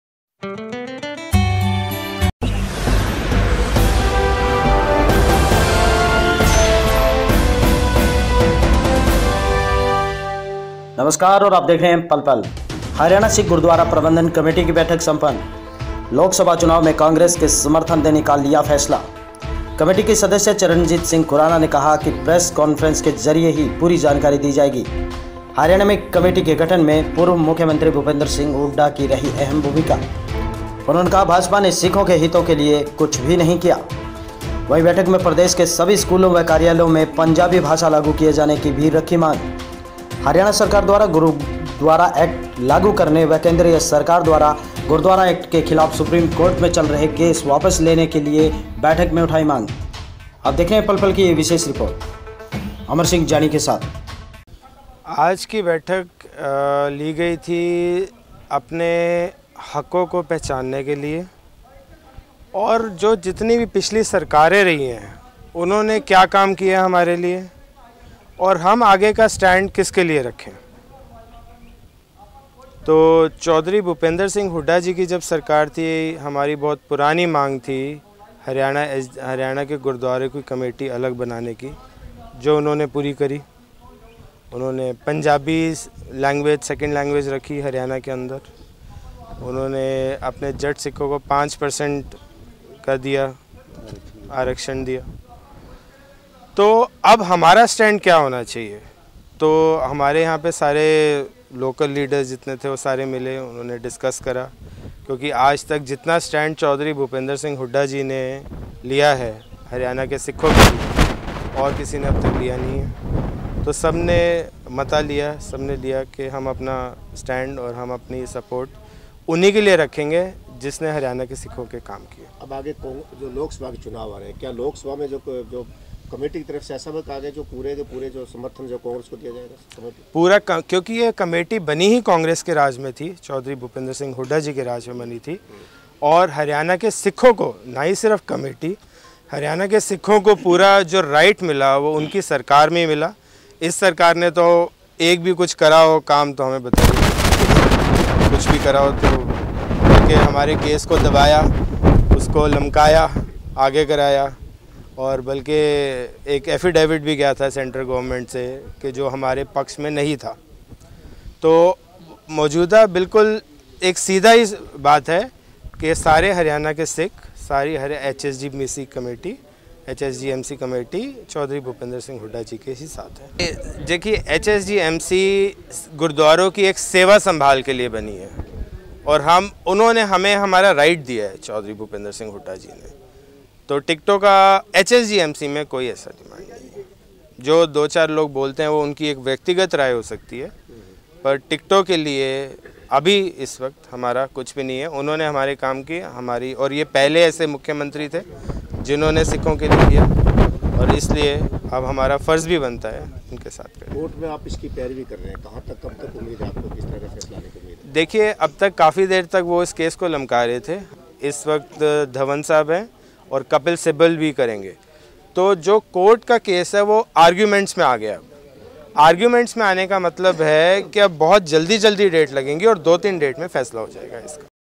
सिरसा सरकार# द्वारा गुरुद्वारा एक्ट के खिलाफ सुप्रीम कोर्ट में चल रहे केस को वापिस लेने की मांग उठी
सरकार द्वारा गुरुद्वारा एक्ट के खिलाफ सुप्रीम कोर्ट में चल रहे केस को वापिस लेने की मांग उठी